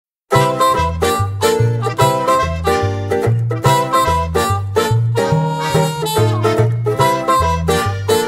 Misi és a vödör
A Vácon előadott szólóest vödrös jelenetéből ollózva.